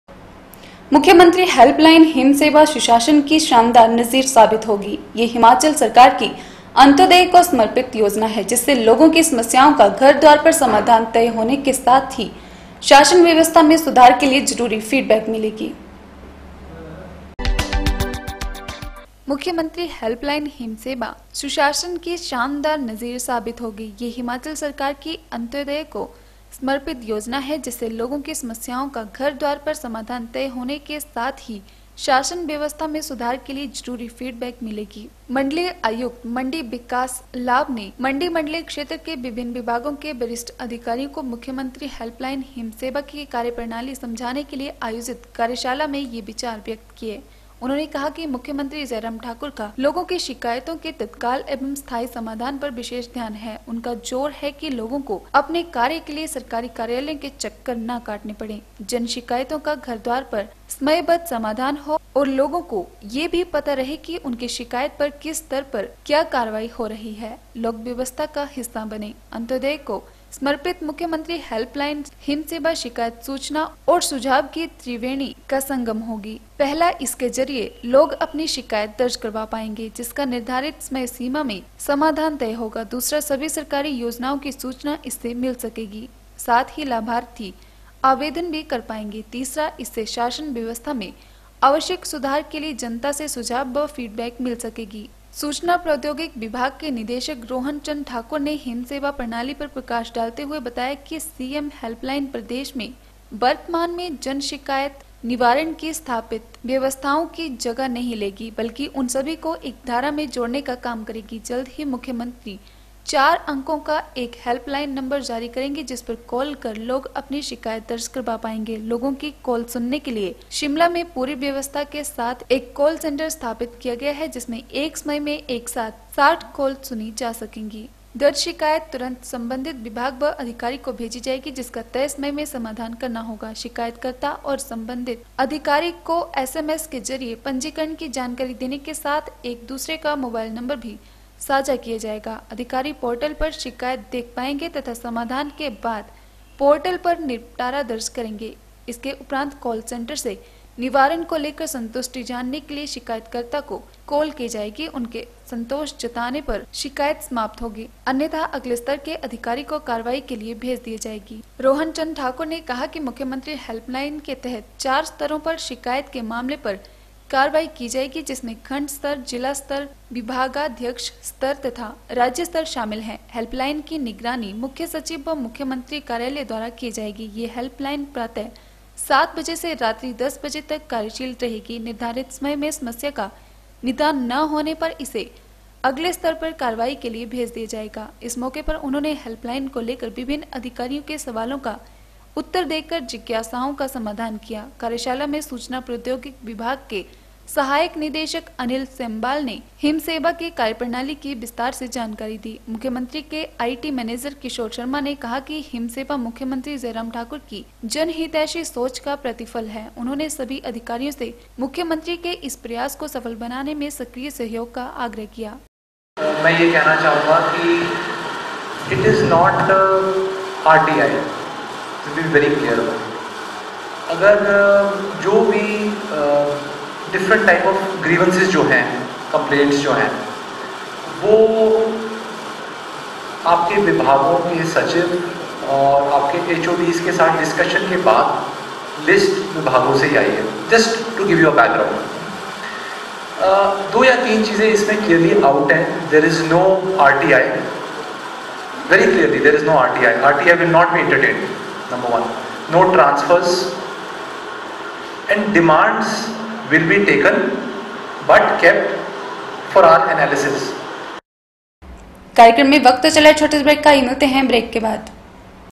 18 JULY N 11 B 3 Chief Minister's helpline 'Himam Seva' is an excellent example of good governance.
मुख्यमंत्री हेल्पलाइन ‘हिम सेवा’ सुशासन की शानदार नजीर साबित होगी। यह हिमाचल सरकार की अंत्योदय को समर्पित योजना है, जिससे लोगों की समस्याओं का घर-द्वार पर समाधान तय होने के साथ ही शासन व्यवस्था में सुधार के लिए जरूरी फीडबैक मिलेगी।